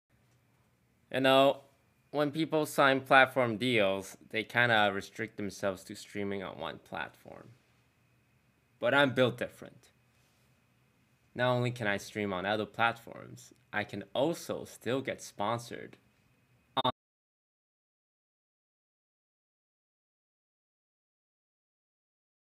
Facebook taking down Toast's stream while he's live on Twitch
All of posted content is AD Free, a subscription and a like would help a lot!
There's no ads on these videos and I do it for fun, if you feel like supporting me,